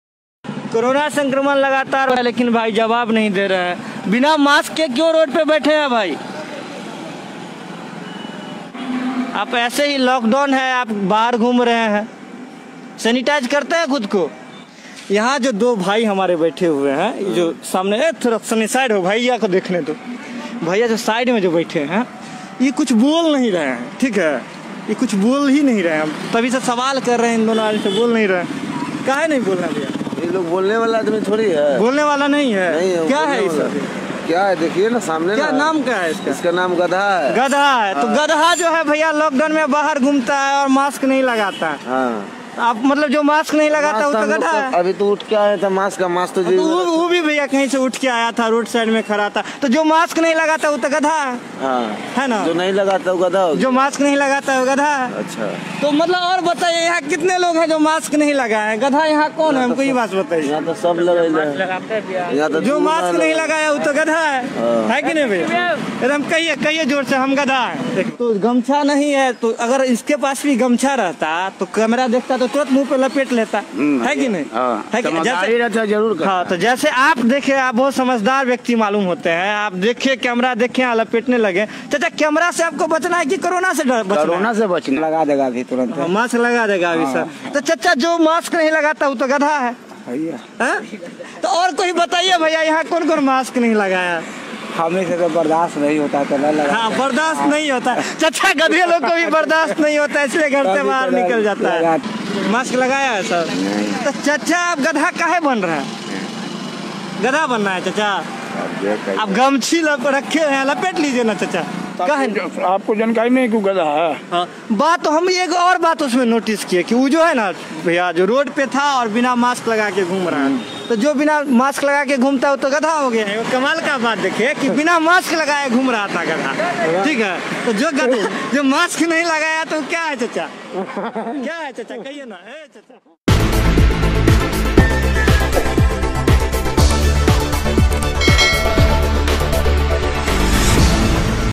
Donkey TV Interview: टीवी रिपोर्टर ने लिया गधे का 'इंटरव्यू', मास्क नहीं लगाने वालों पर कसा तंज,
Donkey TV Interview: टीवी रिपोर्टर ने लिया गधे का 'इंटरव्यू', मास्क नहीं लगाने वालों पर कसा तंज, देखिए मजेदार Video